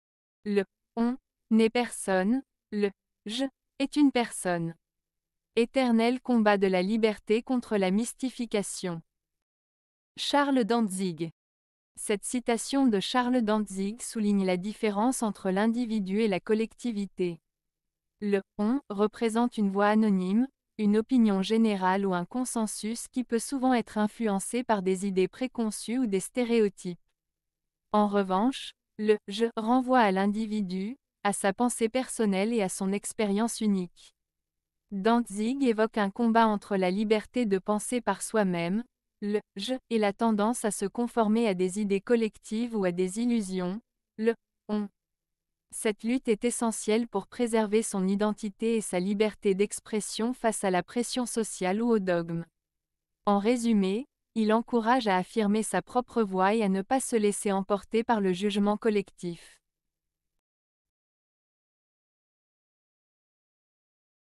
Charles Dantzig - Le « on » n'est personne, le « je » est une personne. Éternel combat de la liberté
Cette citation de Charles Dantzig souligne la différence entre l'individu et la collectivité. Le "on" représente une voix anonyme, une opinion générale ou un consensus qui peut souvent être influencé par des idées préconçues ou des stéréotypes. En revanche, le "je" renvoie à l'individu, à sa pensée personnelle et à son expérience unique.

Dantzig évoque un "combat" entre la liberté de penser par soi-même (le "je") et la tendance à se conformer à des idées collectives ou à des illusions (le "on"). Cette lutte est essentielle pour préserver son identité et sa liberté d'expression face à la pression sociale ou aux dogmes. En résumé, il encourage à affirmer sa propre voix et à ne pas se laisser emporter par le jugement collectif.